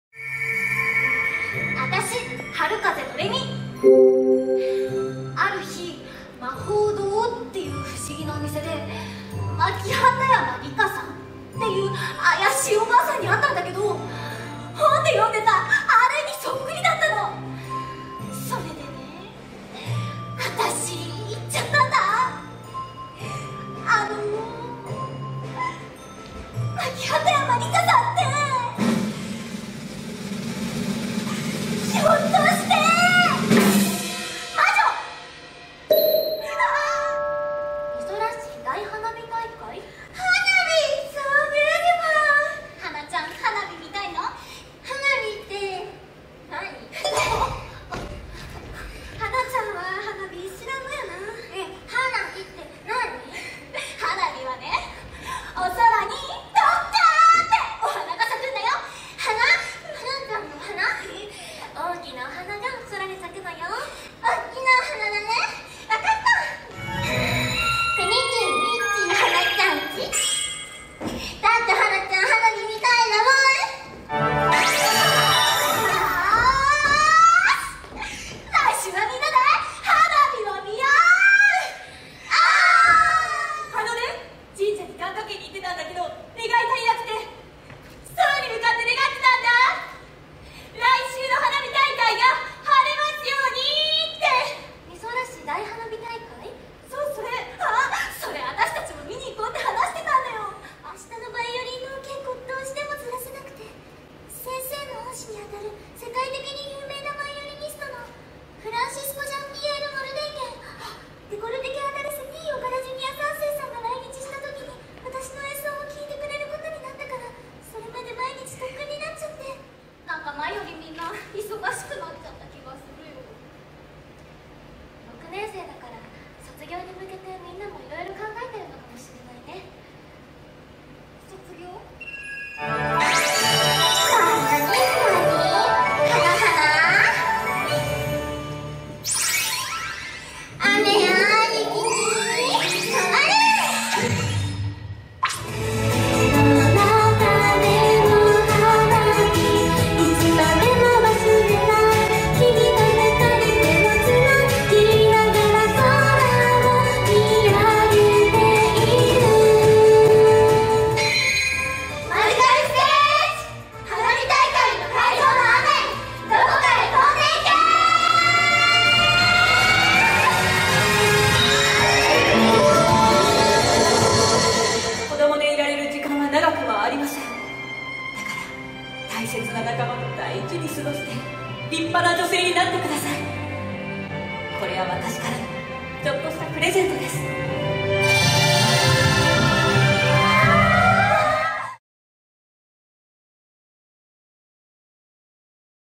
≠ME ACT LIVE「おジャ魔女どれみドッカ～ン！」公開ゲネプロ【コロンチーム】ノットイコールミー／ノイミー│エンタステージ
初心者からツウまで！演劇総合情報サイト『エンタステージ』